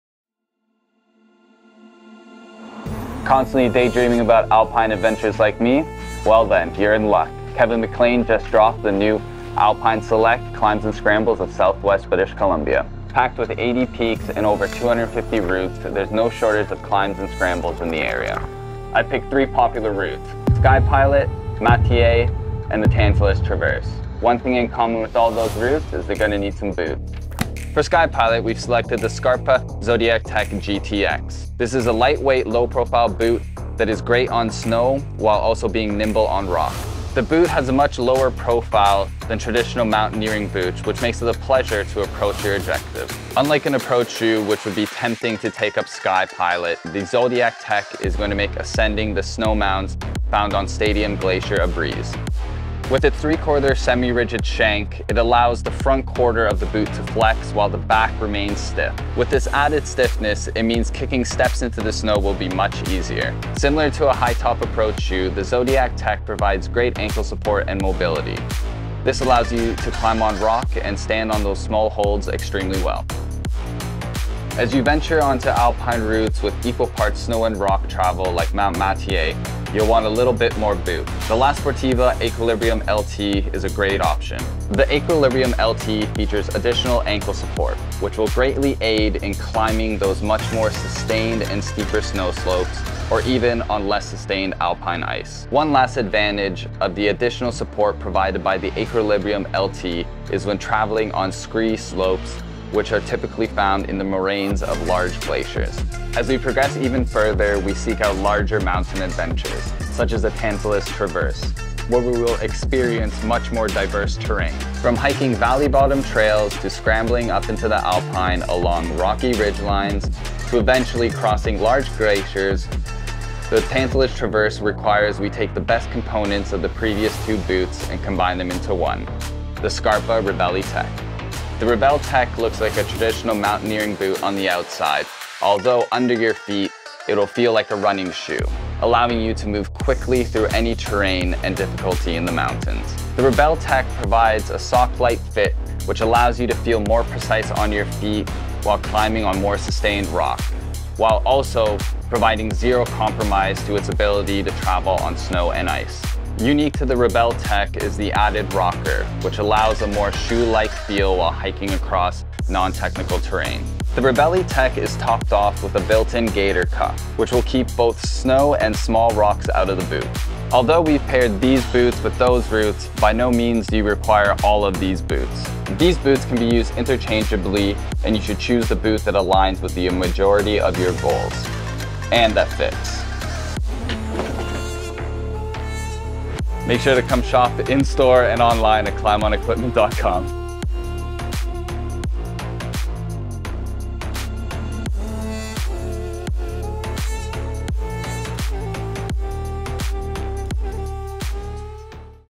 Top Mountaineering Boots for Summer Alpine Climbing
Heading into the mountains for an alpine climbing or scrambling adventure? We have picked out three popular objectives in Southwestern British Columbia and paired three boots that suit those objectives. Having the right boots for the job can greatly help you travel through the mountains.

Headed up Sky Pilot then try the Zodiac Tech from Scarpa
Planning to Summit Mt Matier soon? Check out the Aequilibrium from La Sportiva
Traversing the entire Tantalus range? The Ribelle Tech from Scarpa would be ideal

Whatever you are trying in the mountain we have a selection of footwear from approach shoes to classical mountaineering objectives to hard mixed and ice lines.

This video was filmed on the traditional, ancestral and unceded territory of the Coast Salish peoples – Skwxwú7mesh (Squamish), Tsleil-Waututh & Musqueam First Nations. Thank you for your continued stewardship of these lands.